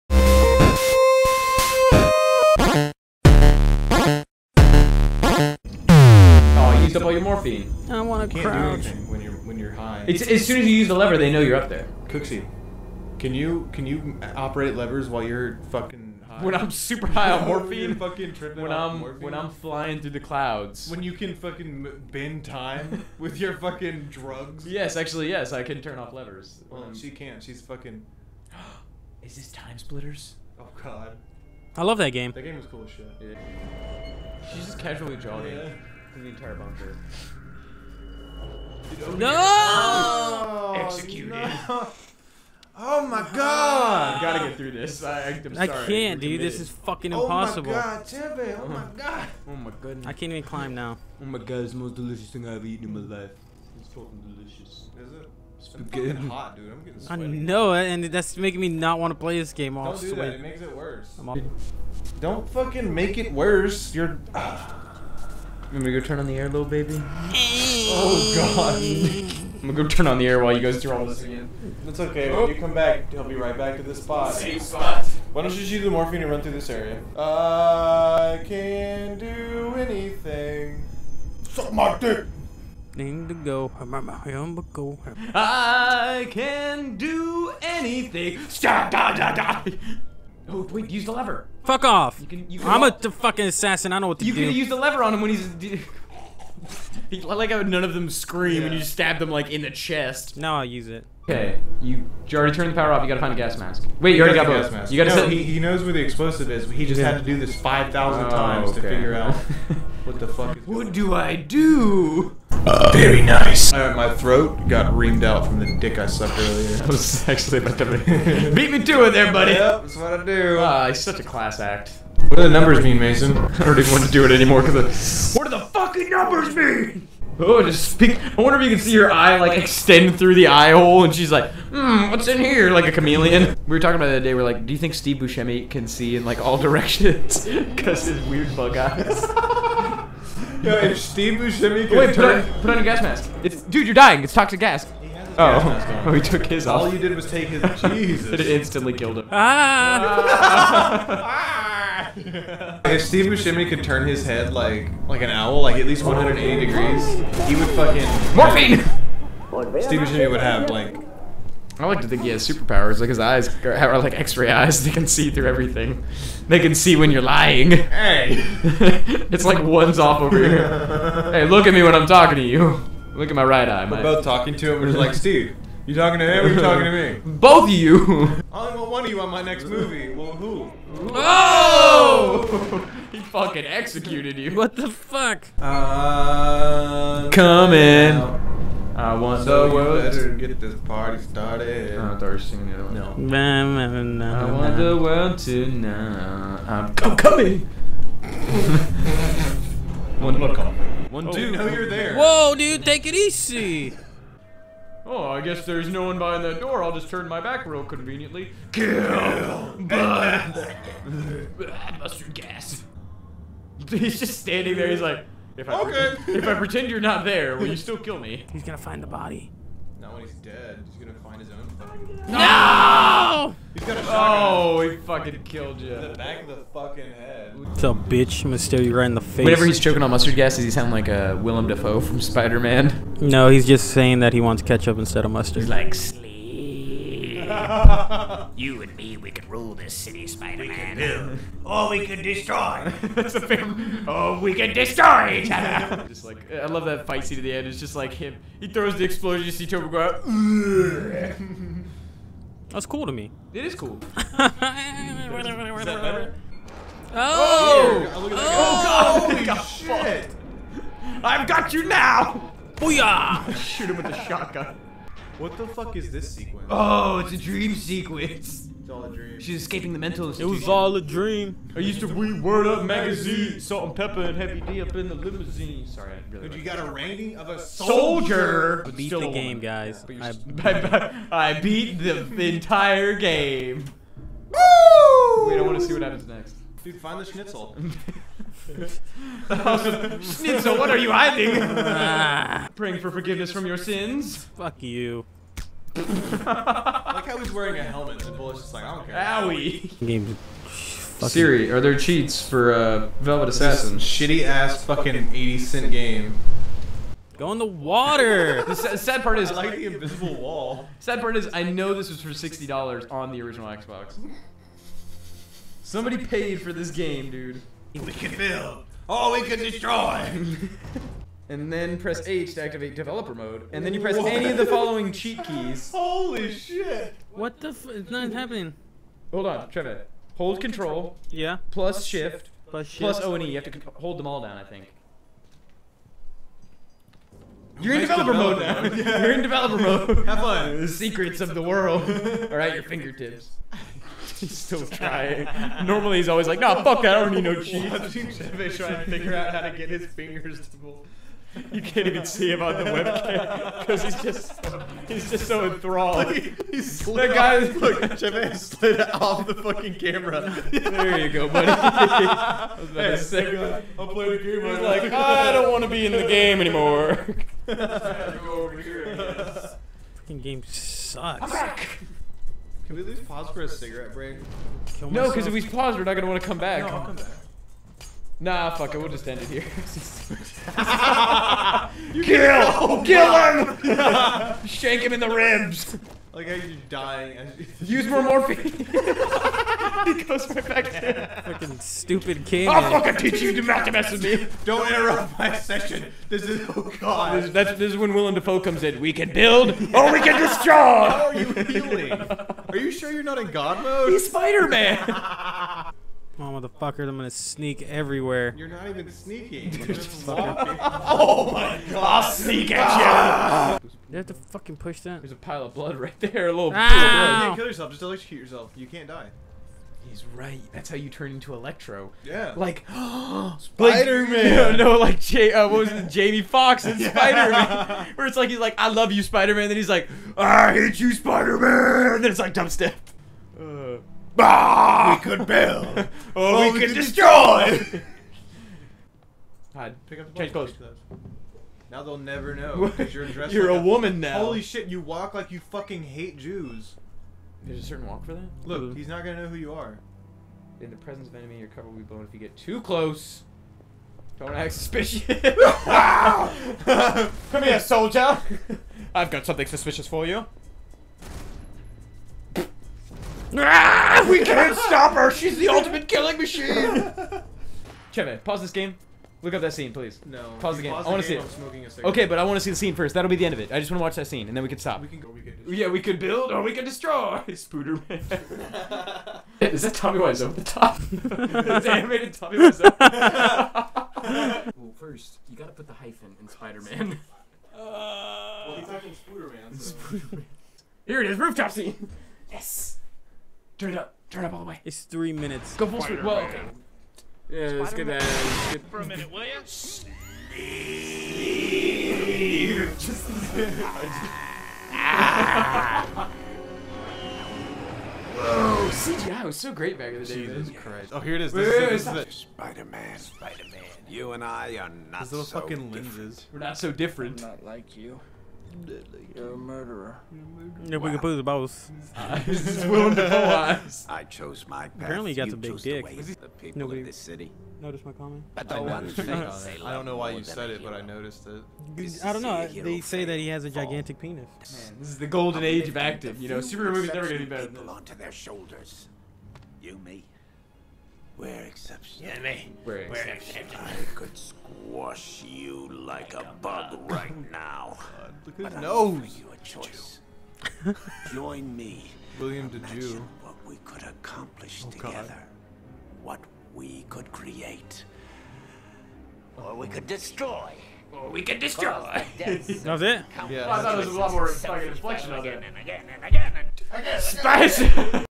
Velvet Assassin-"We could build or DESTROY!"
Spider-man 2002 is a classic film. And Willam DaFoe is our true hero on Meltingpod.
Him and Steve Buscemi with their bug eyes keep us alive.